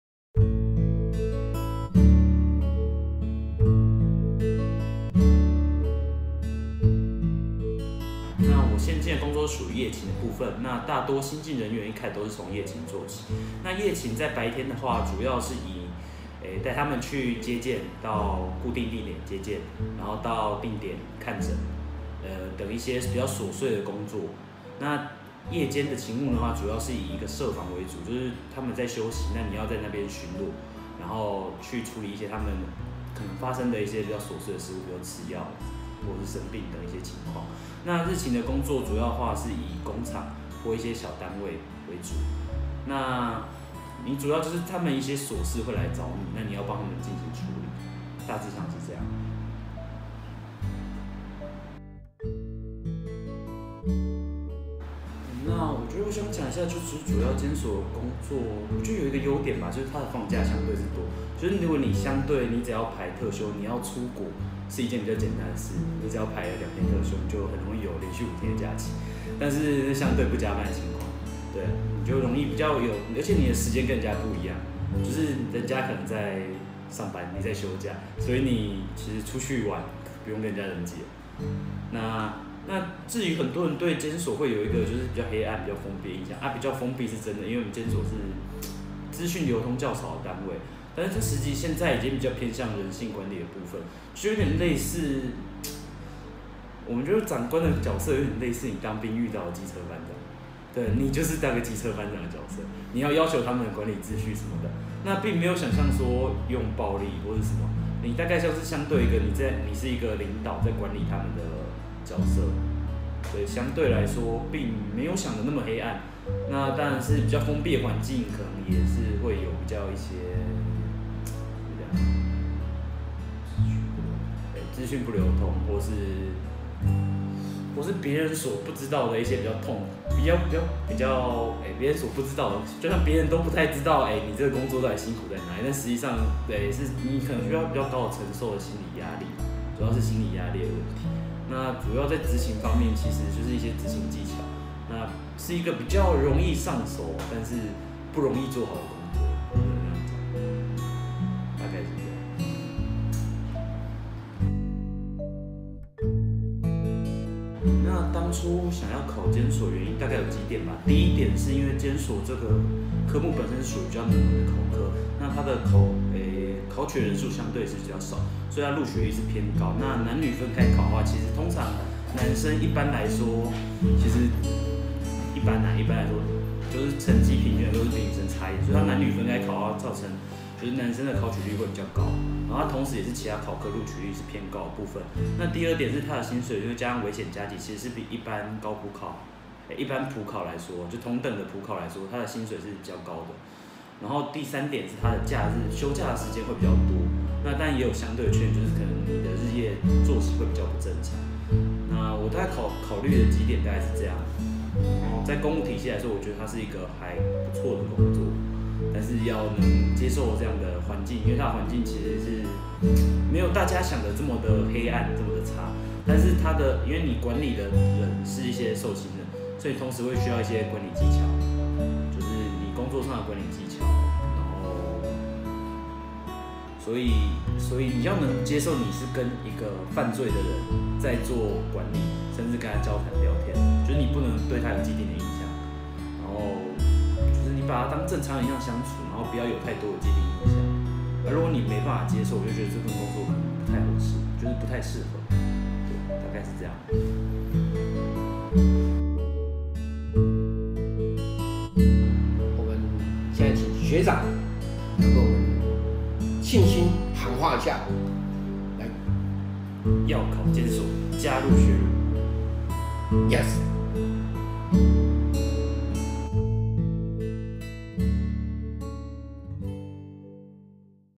保成學儒【公職一日系列分享｜司法｜監所管理員篇】上榜生分享 考上做什麼
準備司法特考埋頭苦讀的艱辛不在話下，然而努力過後總會嚐到最美麗的成功果實！
在監獄工作其實不恐怖？真的是薪水很高但整個月有一半在放假嗎？想知道考上後的實際工作內容嗎？來讓學長說給你聽！
有任何疑問或想分享心得的朋友，歡迎在下方留言哦！

「要考監所管理員，加入保成學儒，YES！！！」

▶監所管理員怎麼考？
【監所管理員輔考資源】